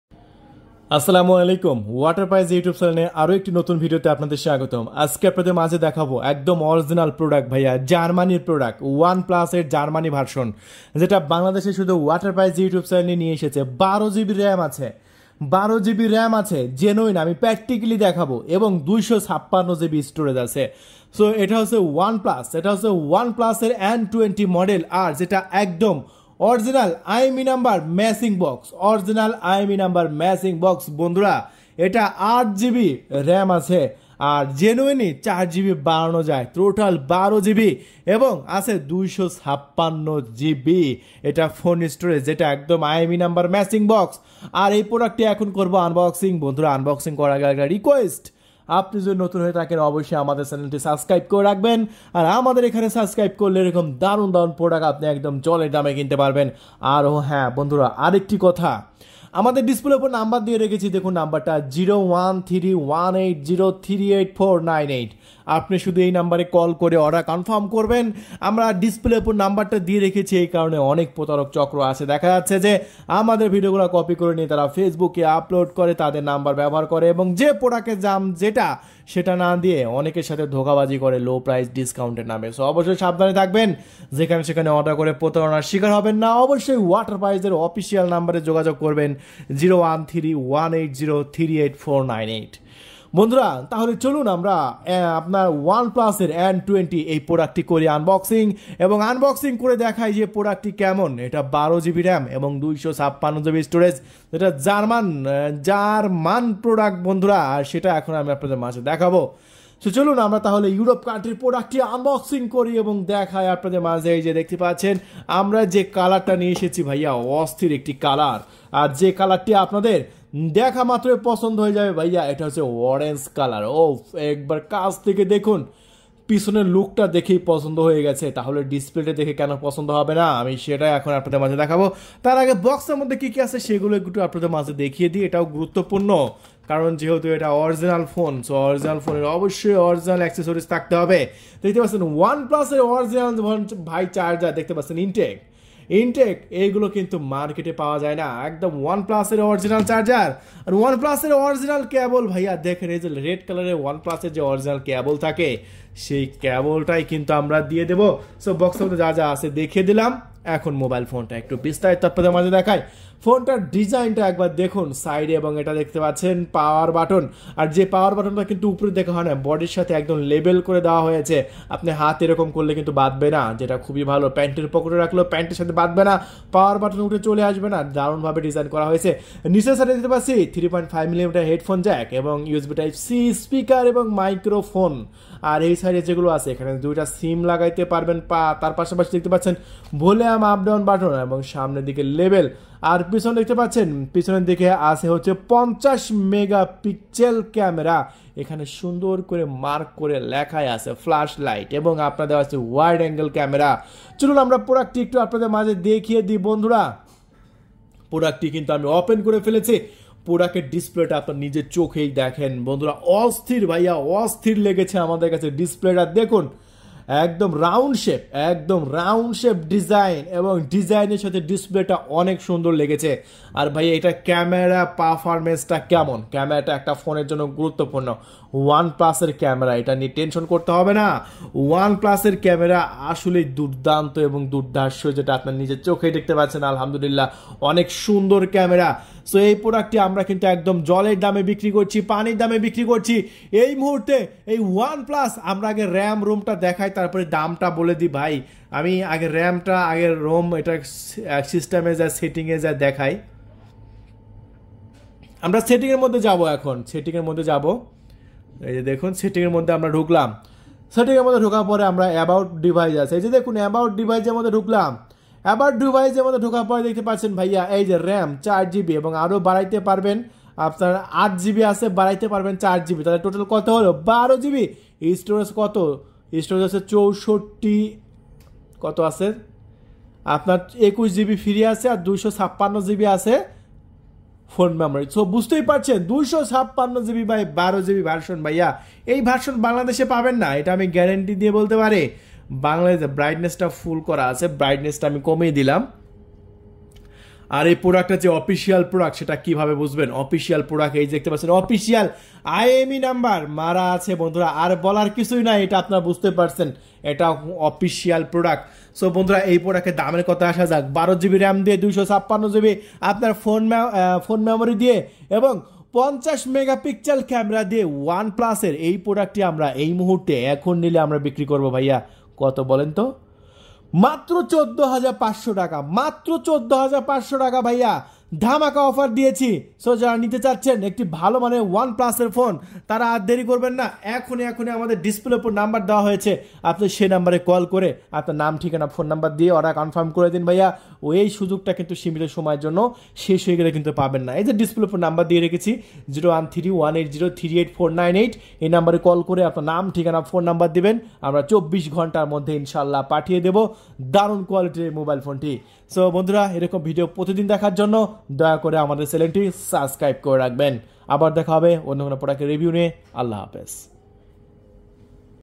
OnePlus Nord N20 SE Unboxing Review !! OnePlus Bangladesh Price || Water Prices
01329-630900 Towfik islam- Managing Director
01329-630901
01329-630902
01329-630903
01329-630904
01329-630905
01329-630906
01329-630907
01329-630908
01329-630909